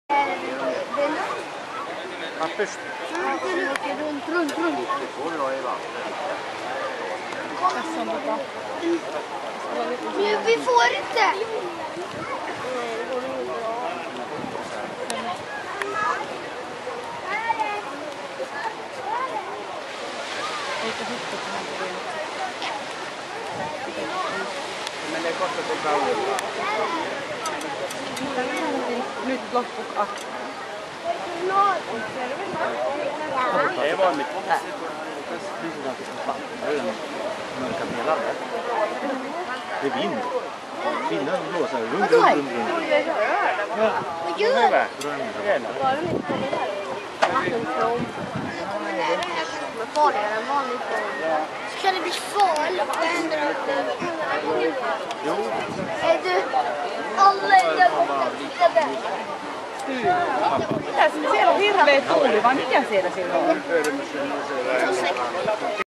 tornado at sea from turkie
we saw 7 tornados at sea from (pascha bay hotel) in turkie  that was very cool.